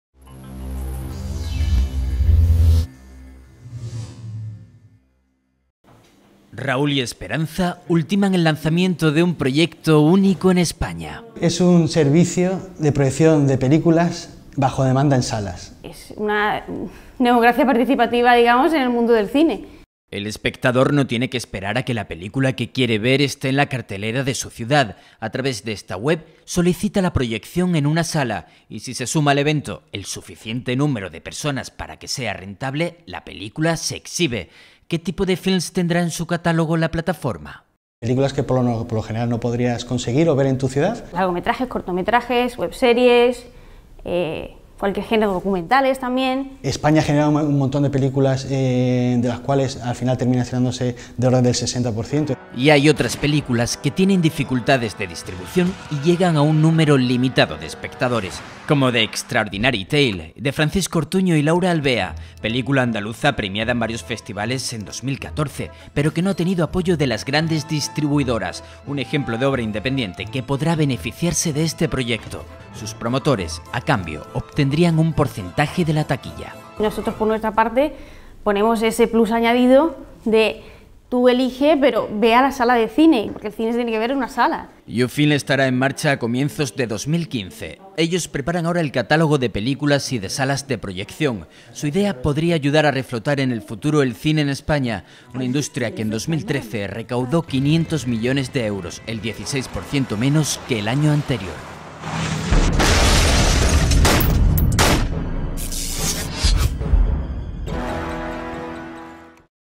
HDL Emprendedores andaluces crean la primera plataforma en España de cine a demanda en salas
Raúl Díez y Esperanza Moreno ultiman el lanzamiento de Youfeelm, una plataforma con la que cualquier espectador puede promover la proyección de la película que desee en una sala de cine. La idea es hacer posible que obras inéditas o con dificultades de distribución lleguen al público: largometrajes, cortometrajes, documentales, webseries... Sus promotores preparan ya el catálogo de películas y de salas que participarán en el proyecto. Tienen previsto que esté en marcha a principios de 2015.